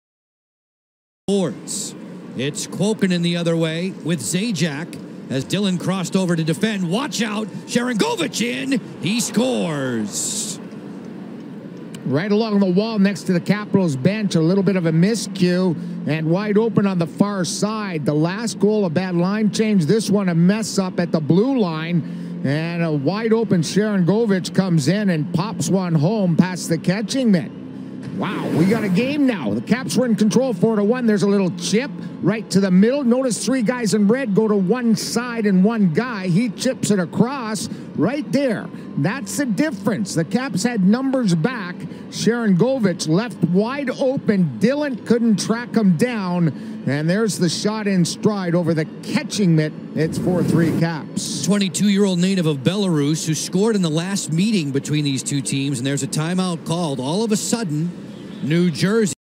3/9/21 Yegor Sharangovich Makes This A One Goal Game In Washington
The New Jersey Devils looks to make this a game as they get to within a goal of tying the Caps thanks to Sharangovich on the quick rush and writer short side on the Caps netminder.